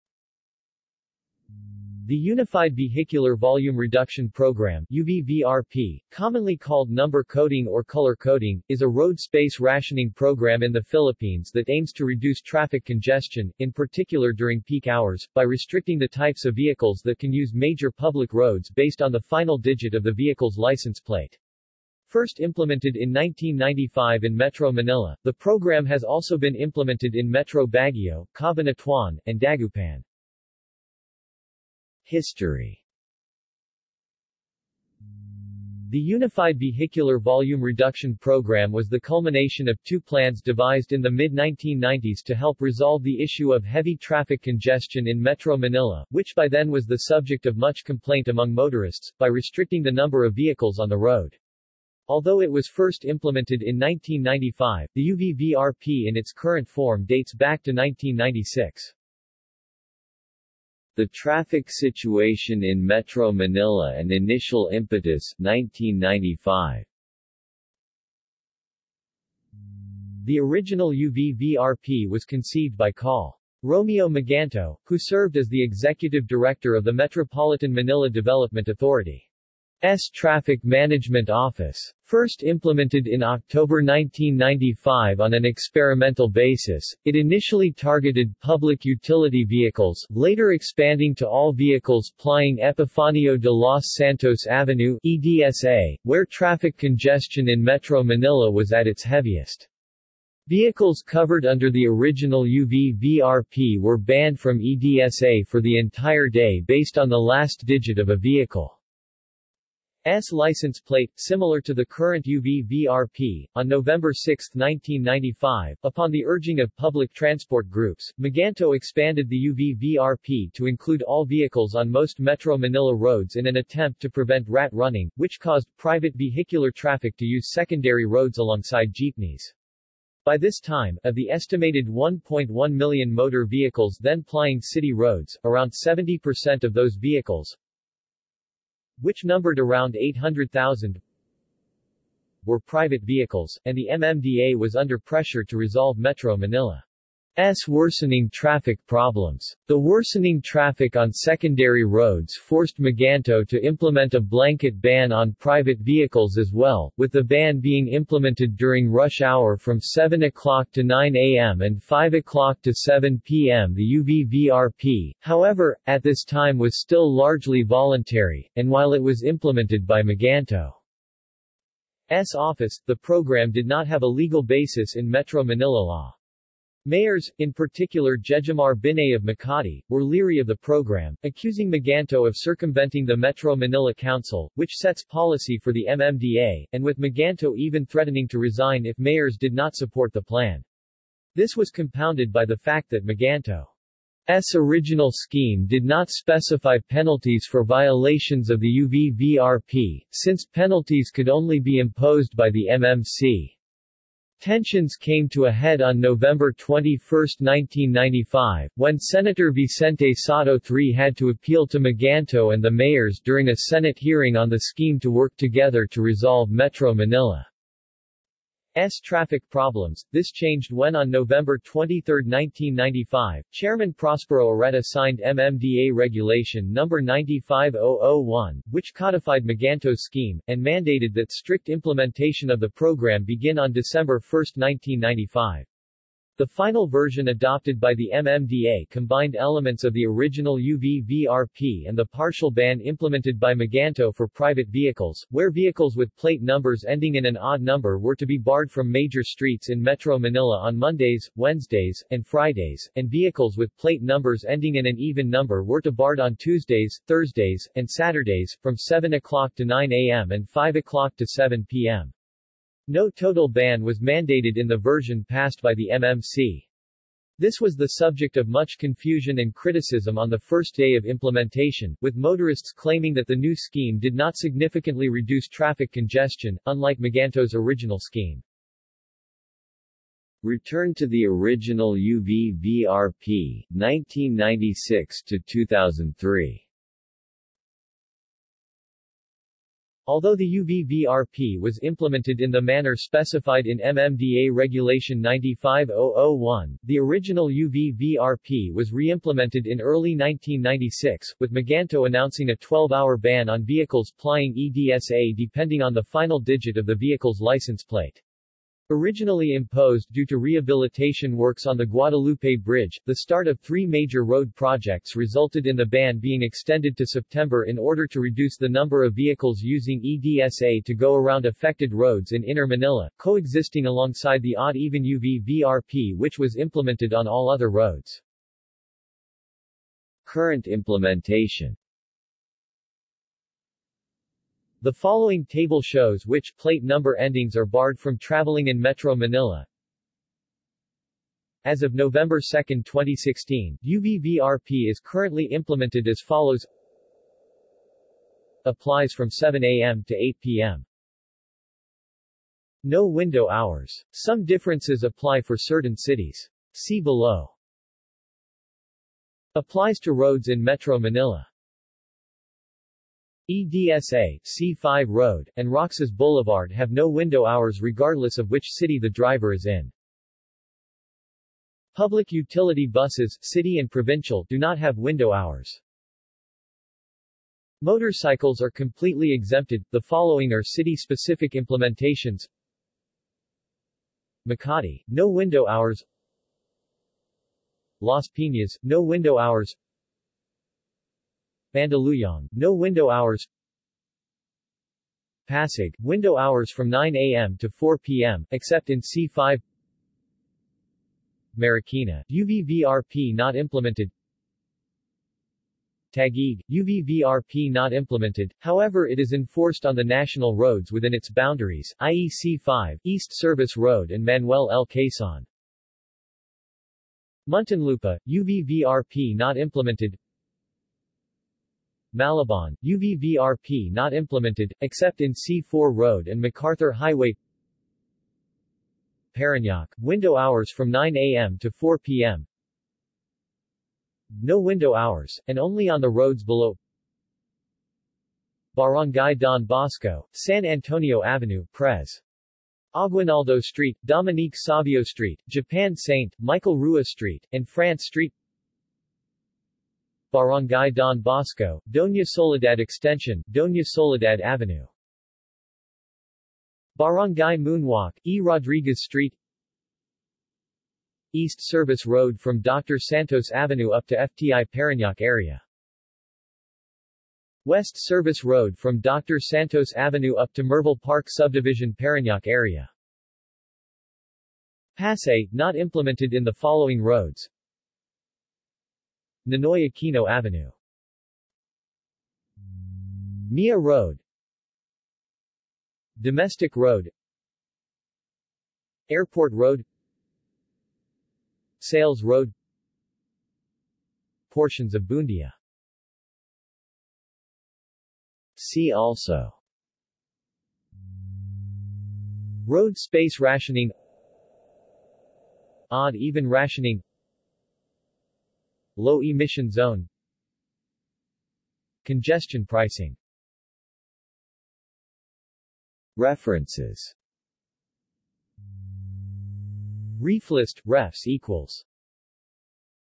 Unified Vehicular Volume Reduction Program | Wikipedia audio article
This is an audio version of the Wikipedia Article:
Unified Vehicular Volume Reduction Program

SUMMARY
The Unified Vehicular Volume Reduction Program (UVVRP), commonly called number coding or color coding, is a road space rationing program in the Philippines that aims to reduce traffic congestion, in particular during peak hours, by restricting the types of vehicles that can use major public roads based on the final digit of the vehicle's license plate.  First implemented in 1995 in Metro Manila, the program has also been implemented in Metro Baguio, Cabanatuan, and Dagupan.